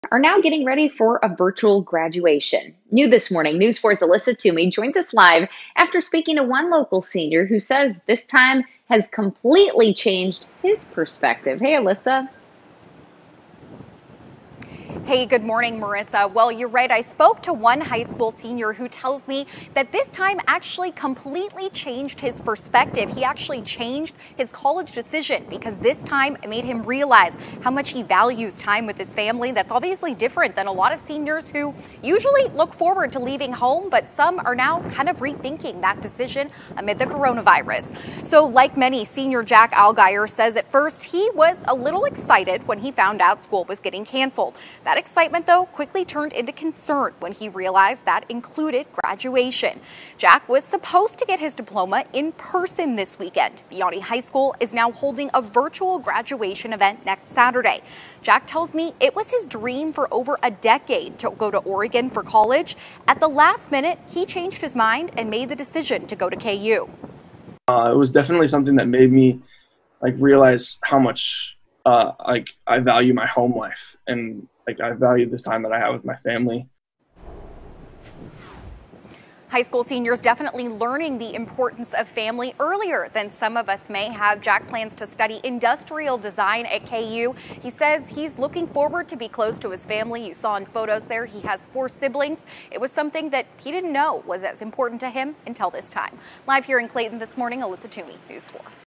St. Louis high school senior says the coronavirus changed his college plans: 'I value time with my
Jake Allgeyer changed his college choice after years of planning because he said this time made him realize how much he values his family.